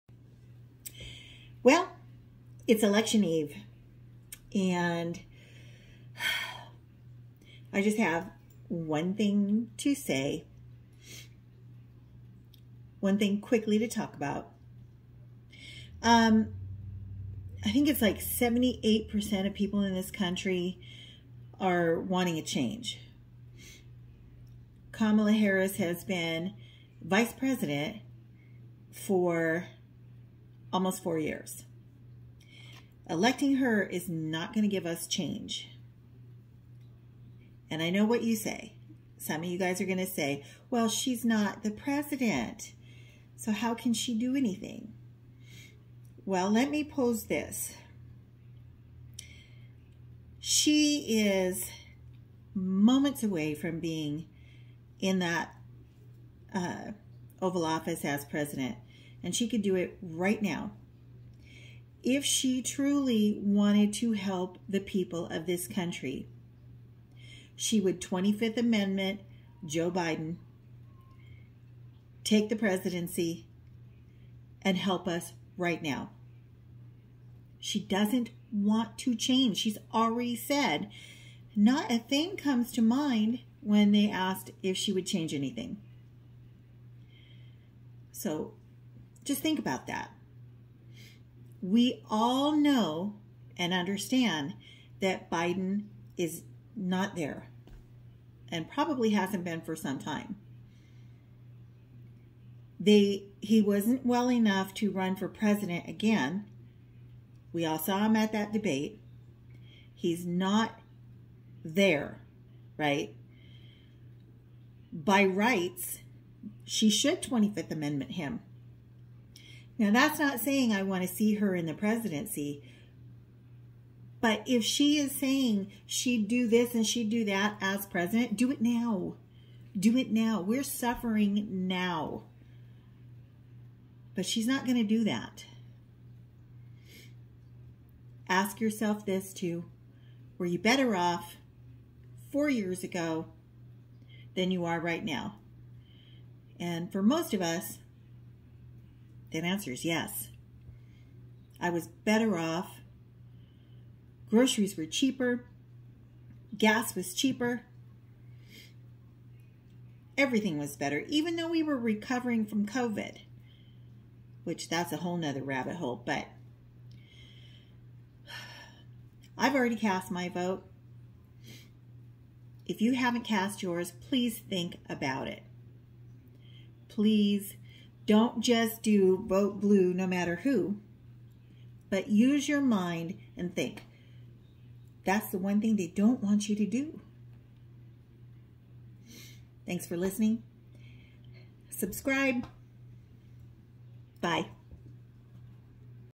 Wrapping Up The Election: Last Minute Musings Before Decision Day
It's election eve and I want you to consider this, if Kamala wanted to help the people she COULD DO IT NOW!  Here are my thoughts.